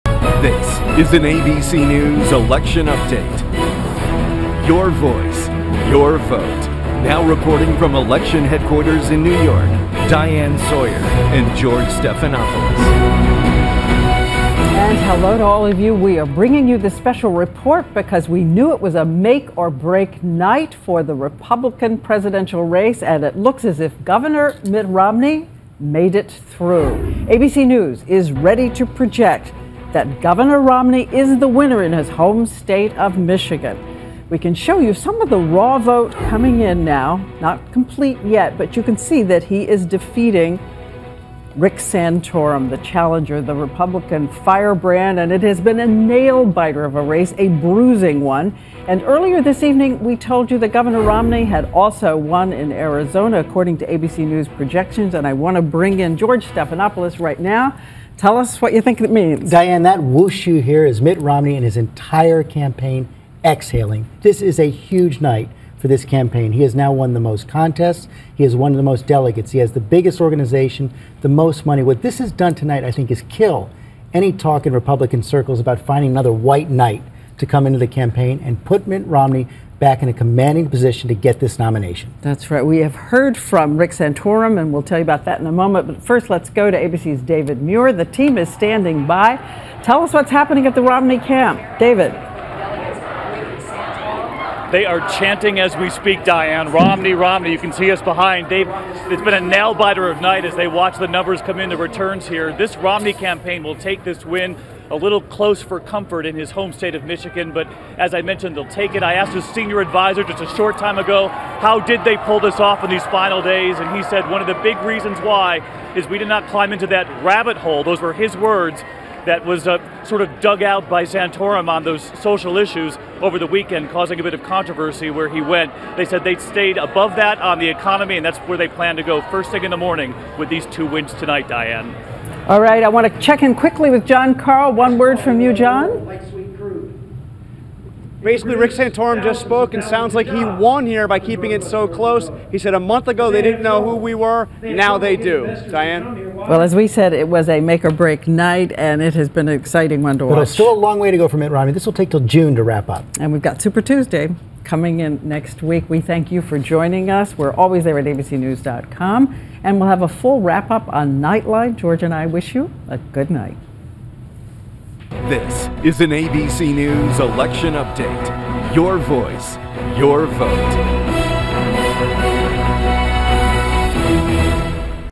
Mitt Romney Projected Winner of Michigan and Arizona Primaries
Mitt Romney won the event, turning back a stiff challenge from Rick Santorum but showing vulnerabilities as well: Santorum won groups he's targeted, including strong conservatives, evangelicals, strong Tea Party supporters and ardent abortion opponents. Union voters, fleeing Romney, also helped Santorum.